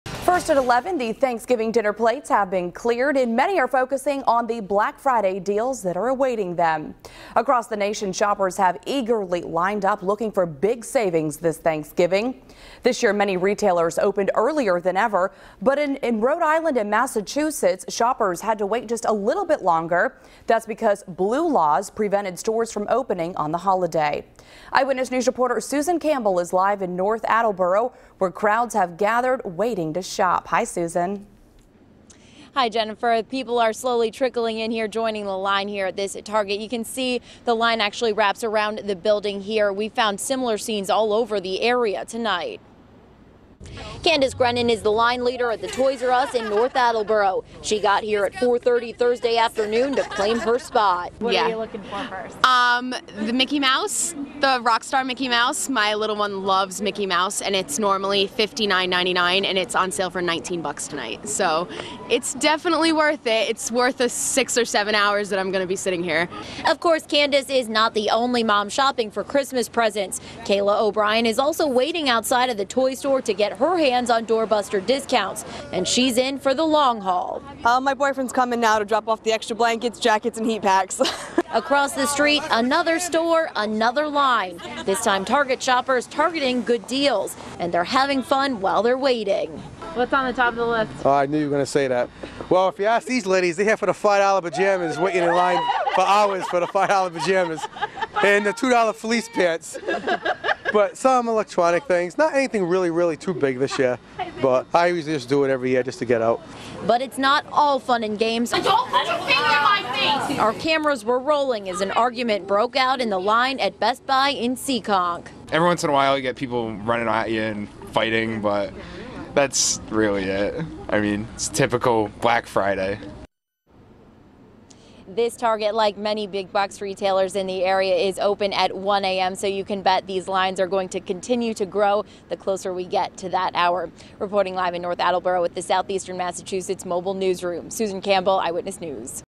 Shoppers line up outside local stores for Black Friday deals.
Shoppers have started lining up outside Southern New England stores this Thanksgiving night looking to score Black Friday deals.